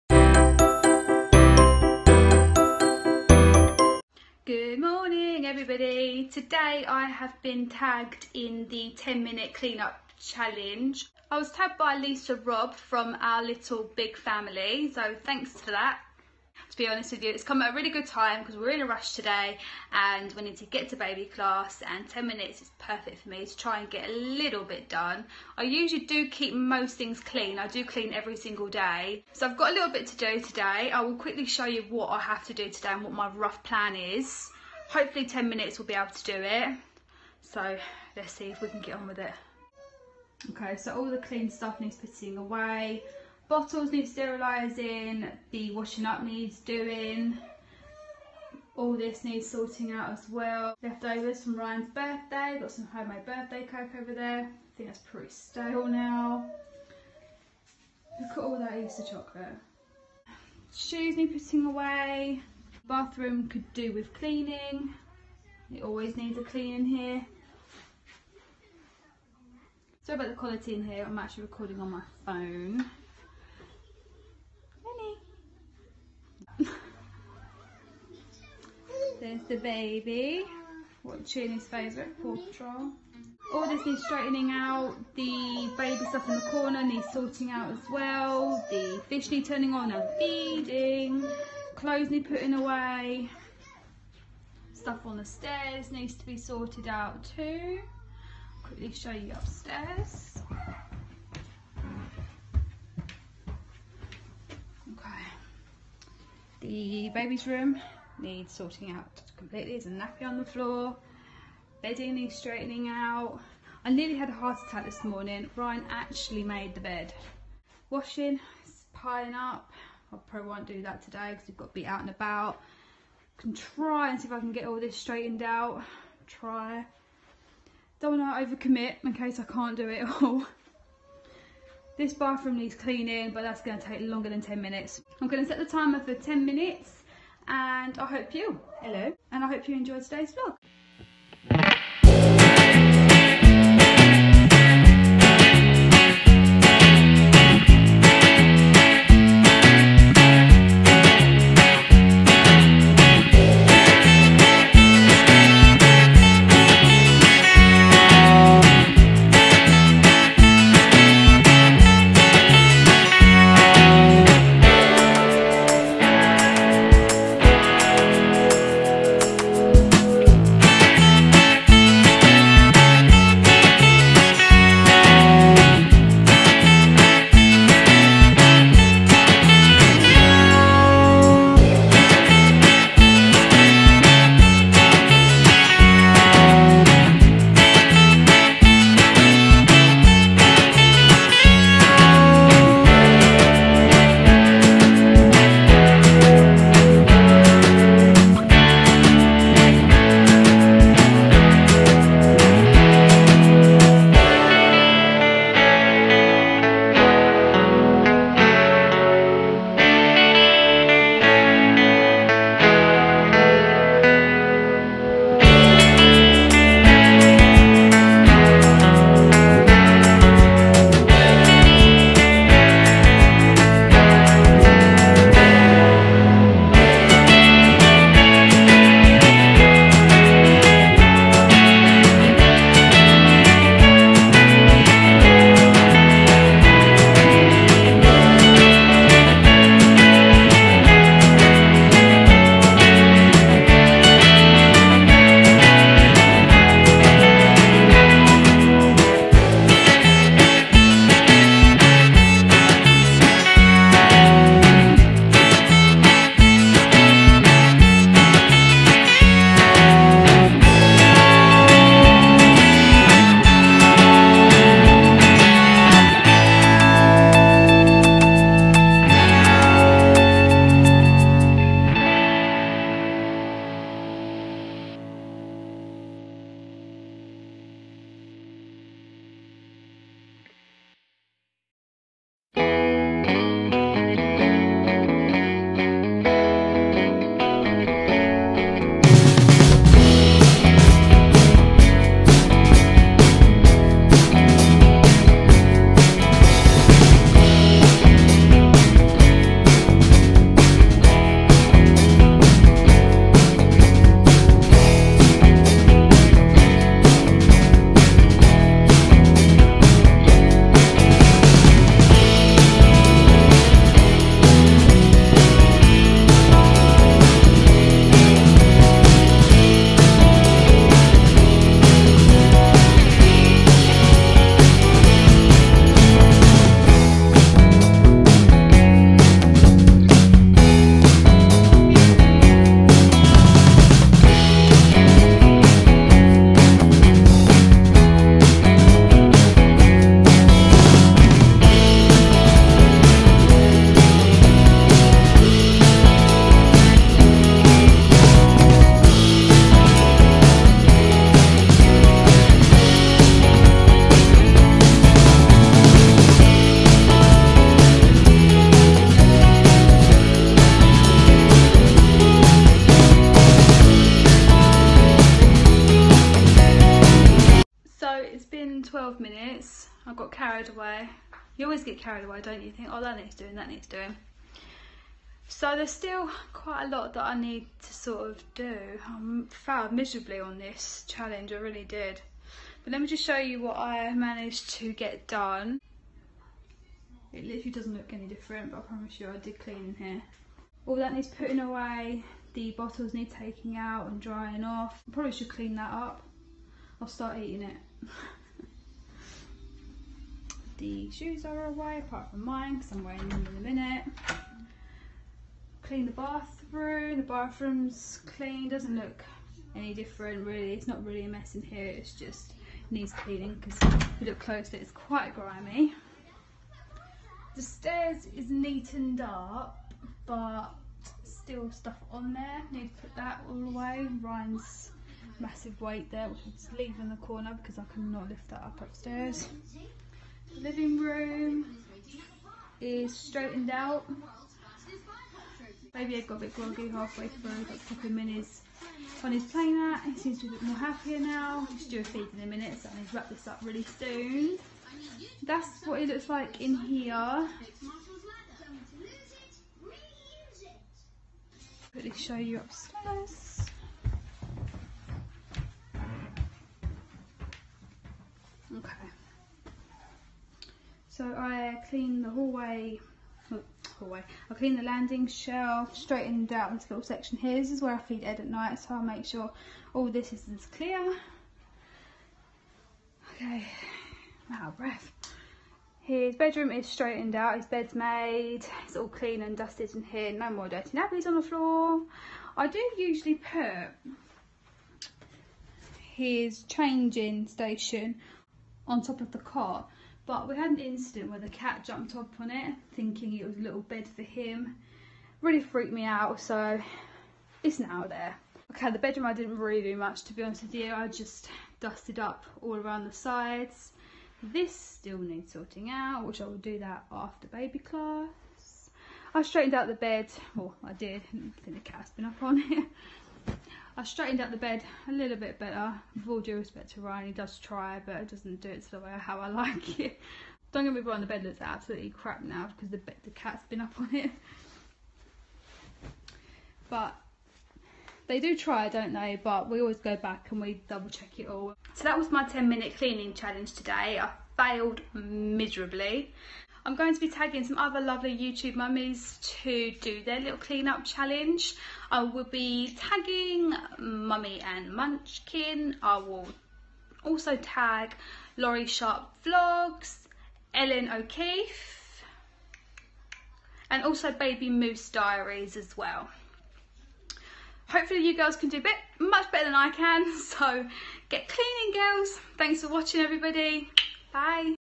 10 Minute Speed Cleaning Challenge Tag
I was tagged to do a 10 minute cleaning challenge, I FAILED! I didn't manage to clean what I aimed to get done, but at least some of it is done!

GET CLEANING LADIES!